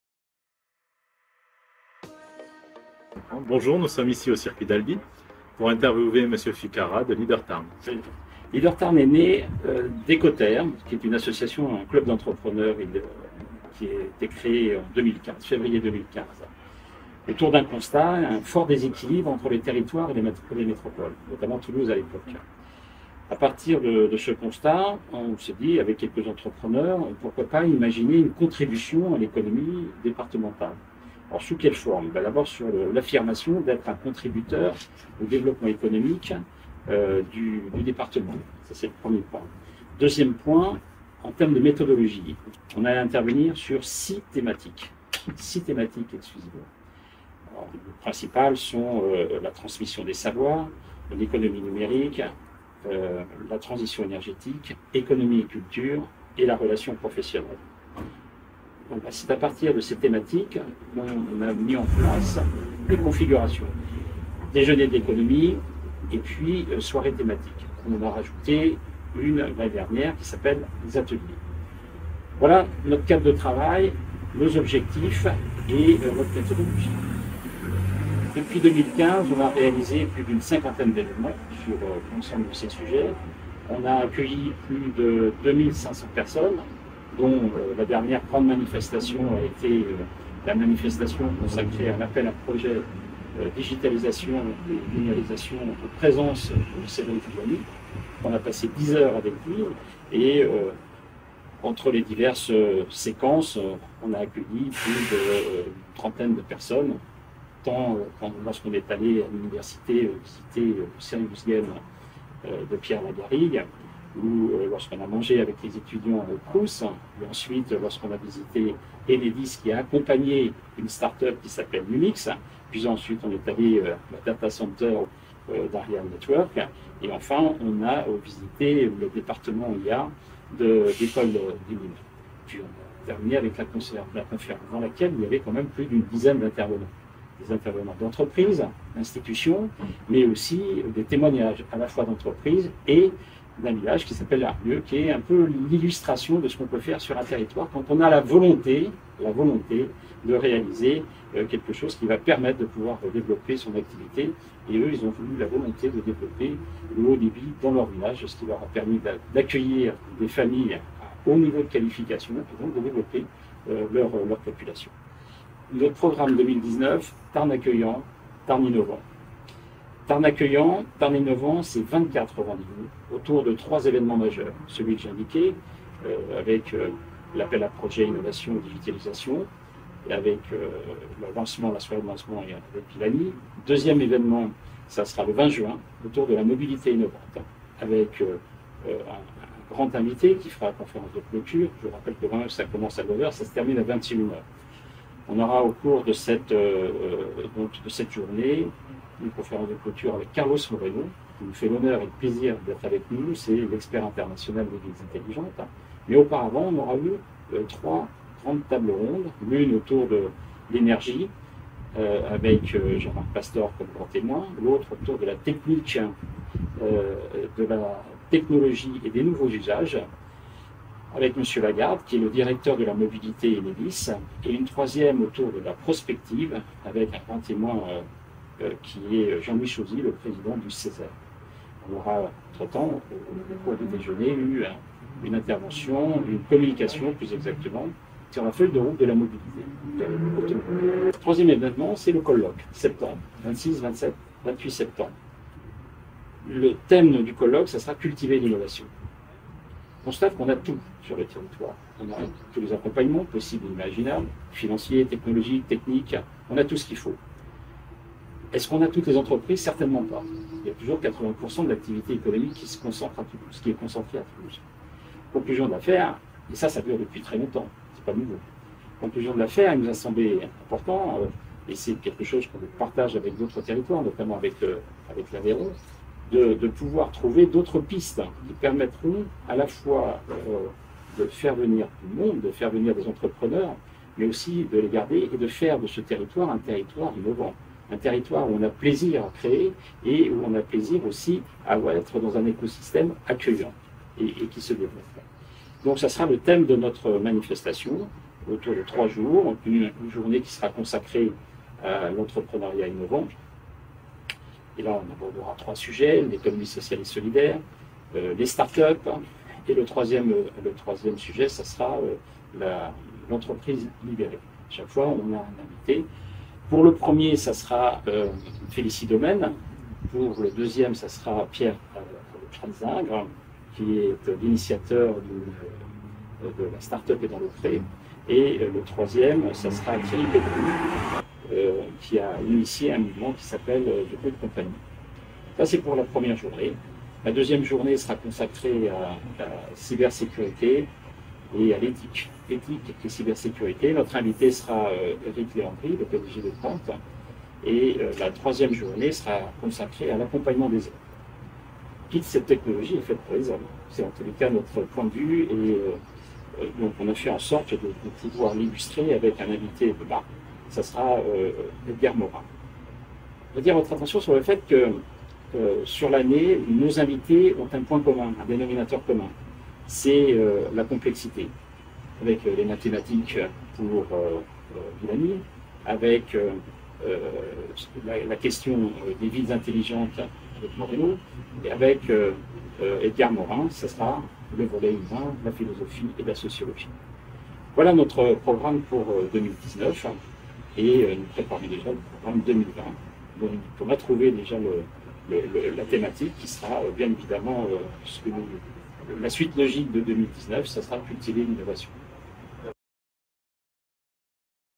Fredy Ficarra nous présente Leader Tarn et la première rencontre de la mobilité innovante.
C'est sur le circuit d'Albi, où se déroulera la prochaine rencontre de la mobilité innovante, que Fredy Ficarra, Président de Leader Tarn, s'est exprimé  pour nous présenter l'action, les objectifs et les prochains grands évènements de Leader Tarn. Né d'un constat, un fort déséquilibre entre territoires et métropole, Leader Tarn s'engage pour contribuer au développement économique du département du Tarn. 6 thématiques, dont la transmission des savoirs, l'économie numérique, la transition énergétique... et une méthodologie d'échanges autour de ces thèmes. Depuis 2015,  plus d'une cinquantaine d'évènements ont été organisés, et près de 2500 personnes ont participé à ces rencontres, dont la dernière manifestation à l'école des Mines d'Albi Carmaux, avec le lancement de l'appel à projet" digitalisation et numérisation" lors d'une journée avec Cédric Villani, que TVDICI a, par ailleurs, relayée. Le programme 2019 s'oriente autour d'une idée forte : Tarn accueillant, Tarn Innovant. 24 rendez vous autour de 3 évènements majeurs, celui cité plus haut, mais aussi le 20 Juin, la première rencontre de la mobilité innovante, avec Carlos Moreno comme invité d'honneur et de grandes tables rondes : l'une autour de l'énergie avec Jean -Marc Pastor comme grand témoin, l'autre autour de la technologie et des nouveaux usages avec Dominique Lagarde, directeur de la mobilité Enedis et la troisième avec le président du Ceser, Jean-Louis Chauzy,  sur la prospective.
3 ème grand évènement, le colloque : 26, 27, 28 septembre, sur le thème "Cultivez l'innovation" . Première journée consacrée à l'entreprenariat innovant avec  3 sujets : l'Economie Sociale et Solidaire, les start-up, et l'entreprise libérée. Avec pour intervenants respectifs : Félicie Domène, Pierre Alzingre et Thierry Pédeloup. La seconde journée sera consacrée à la Cybersécurité et à l'éthique : l'invité sera Eric Léandri, le PDG de Qwant, et la troisième journée sera consacrée à l'accompagnement des hommes, avec comme invité Edgar Morin.
En 2020, la suite logique du programme sera " Cultiver l'Innovation"...  Un programme qui promet encore de beaux temps forts et d'échanges.